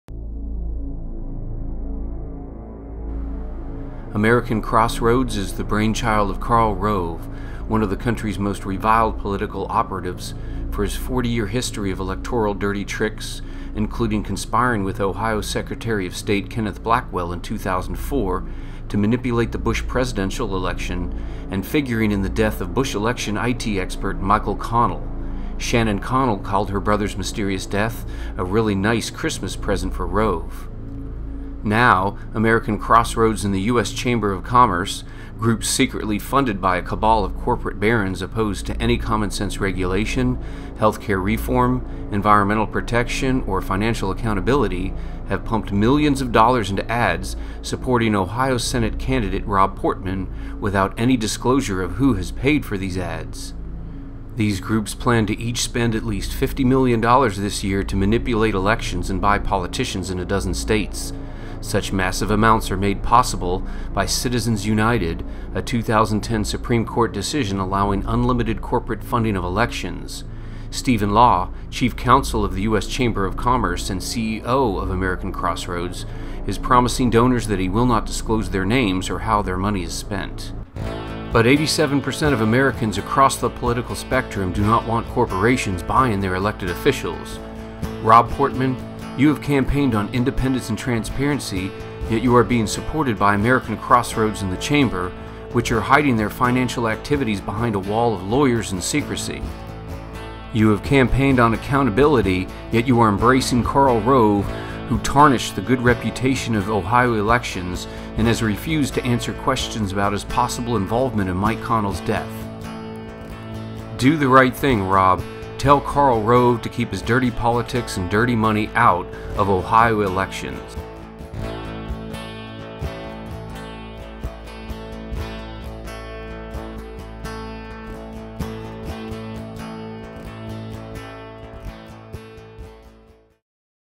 AmericanCrossroadsWatch.Org - Rob Portman Should Reject Rove's Dirty Tricks
- AmericanCrossRoadsWatch.Org exposes the dirty tricks of Karl Rove, the US Chamber of Commerce, and the billionaire barons that are undermining American elections by secretly funneling money through the 501c4 astroturf organization known as "American Crossroads". By using a 501c4, the names of the donors are hidden to the public, skirting elections laws. They are buying politicians and using their big business cash to influence elections. They are currently running ads in Ohio in support of Portman with their secret funds. We call on Rob Portman to denounce this practice, and reject the support of this group.